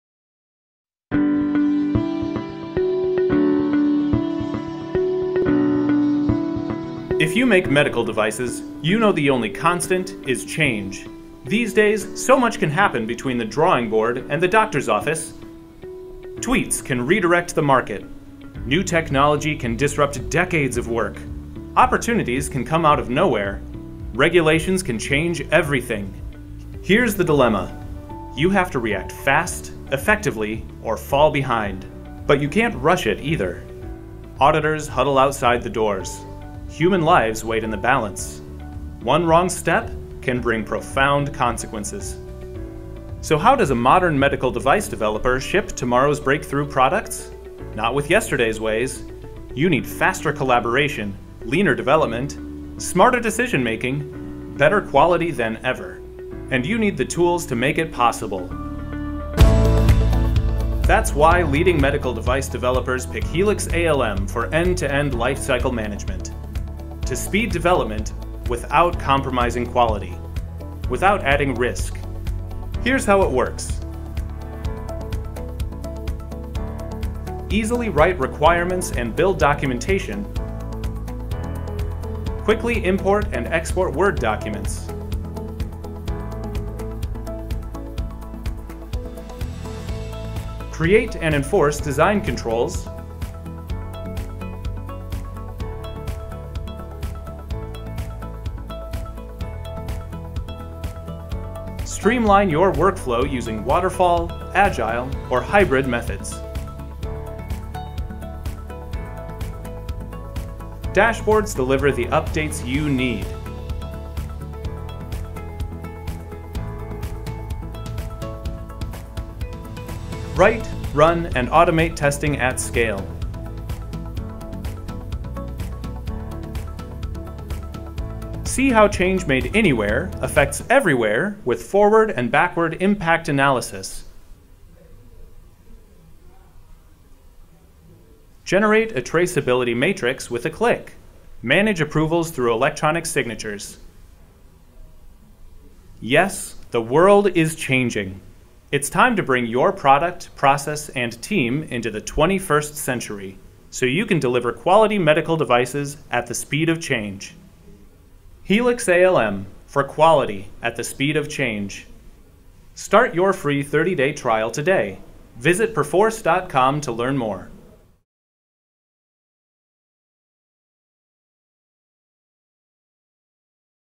Solving the Dilemma of Modern Medical Device Development
Medical device development continues to change. How do medical device developers stay ahead? See how in this short video about Helix ALM.